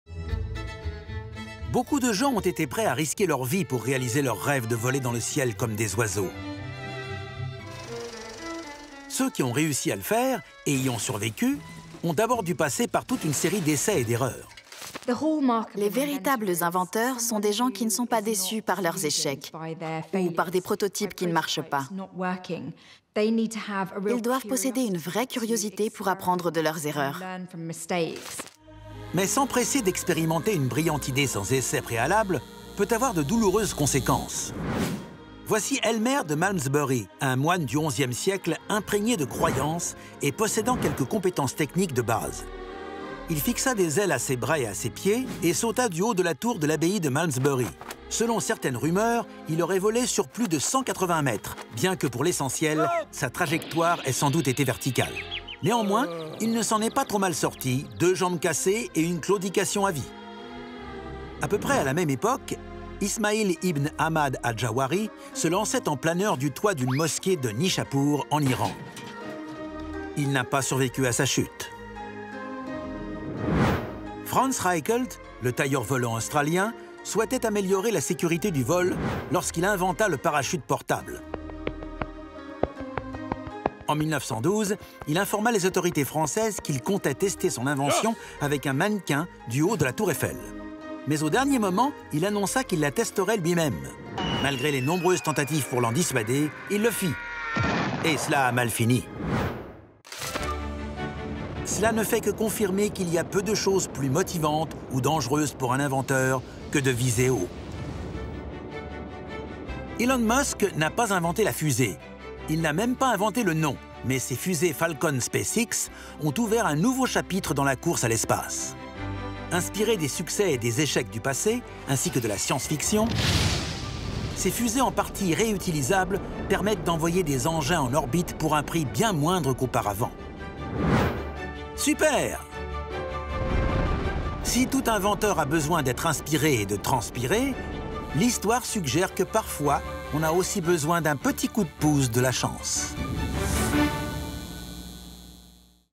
Ces gens se sont jetés dans le vide en essayant de prouver que leur invention fonctionnait
Les inventions géniales reposent souvent sur une ribambelle d'échecs. Mais pour perfectionner son œuvre, encore faut-il rester en vie.